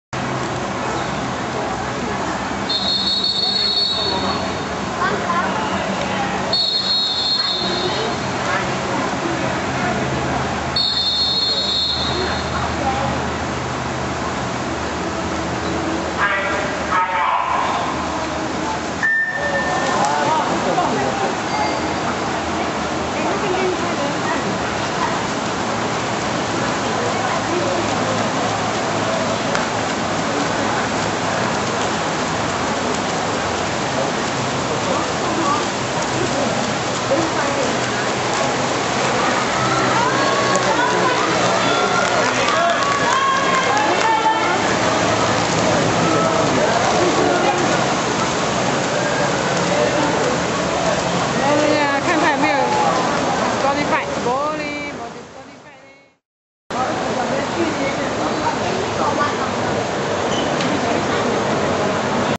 Inter School Swimming Nationals 09 - C Boys 50m backstroke final
The final race #09-01 was swum at the Singapore Sports School on 28 April (Tue) morning.

Placings:
1. Syed Illyas (RI) - 29.65s
2. Koh Quan Ren (RI) - 31.66s